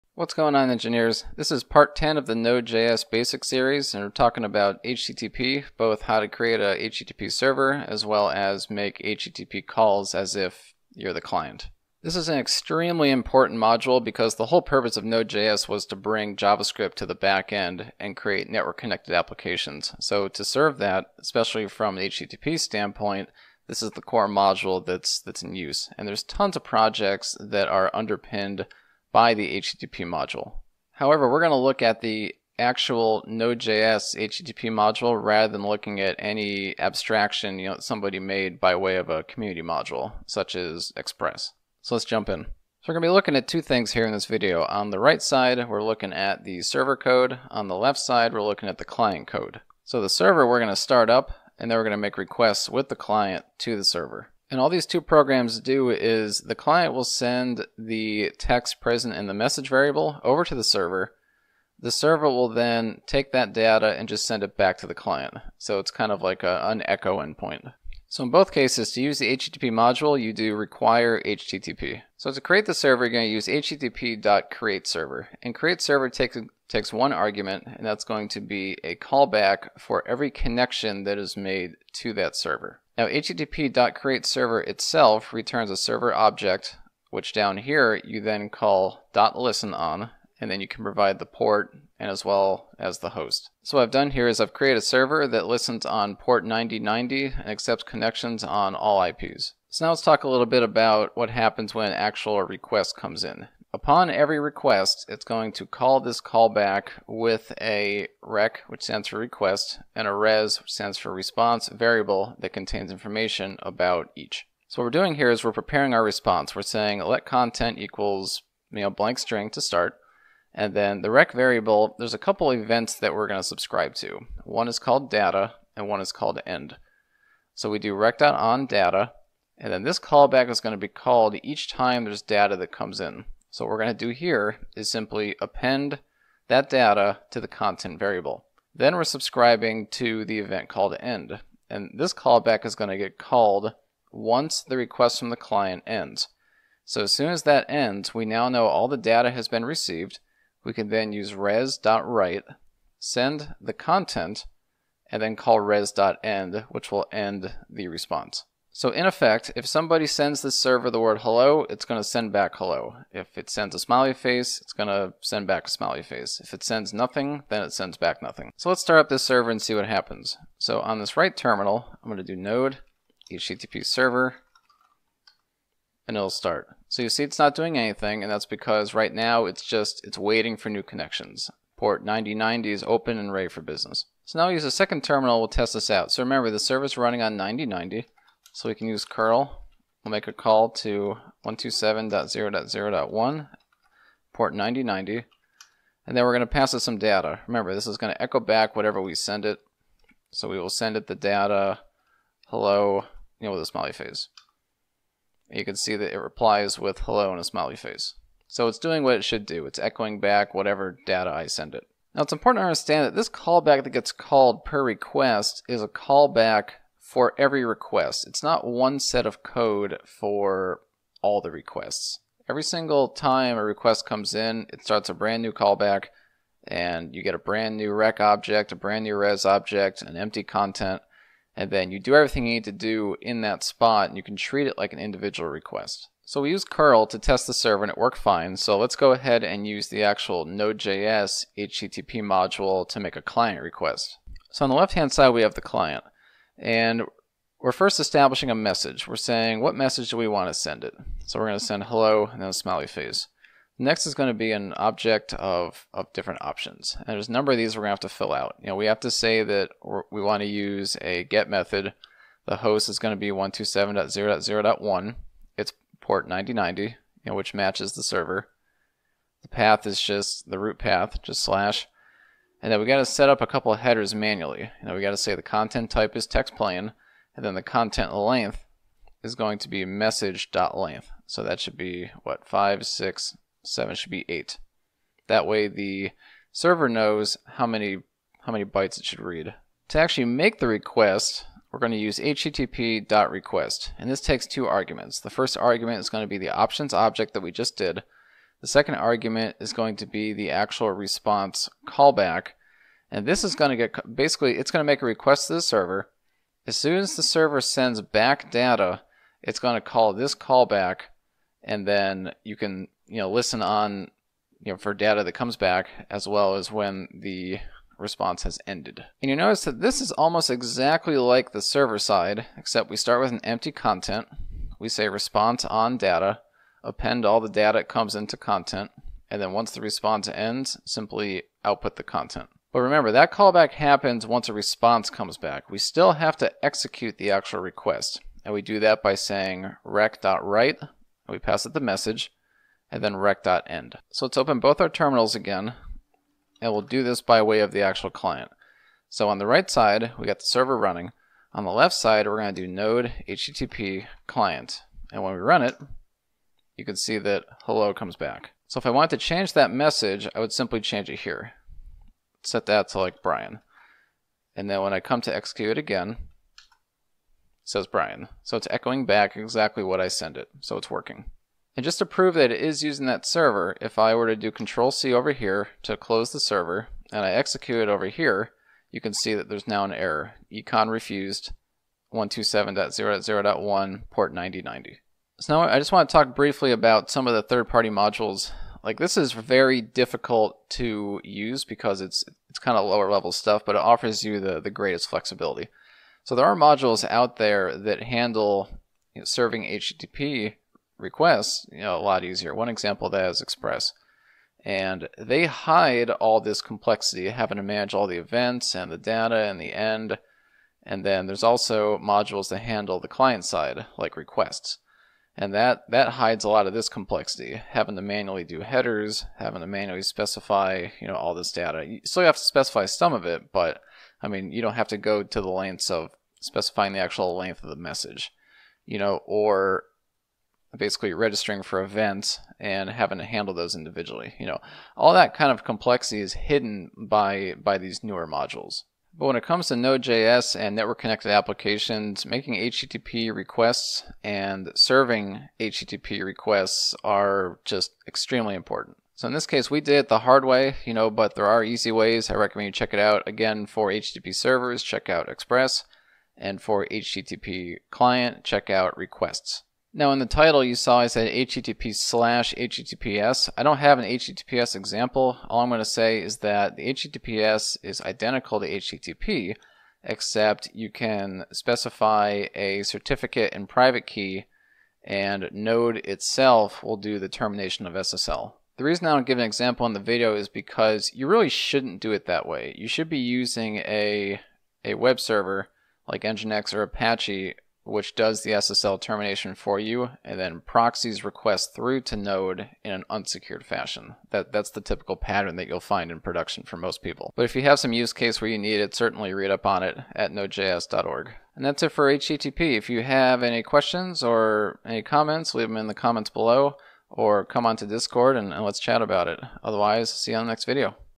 HTTP - Node.js Basics Part 10
The HTTP module is an extremely important module for network connected Node.js applications.

Sincerely,
Engineer Man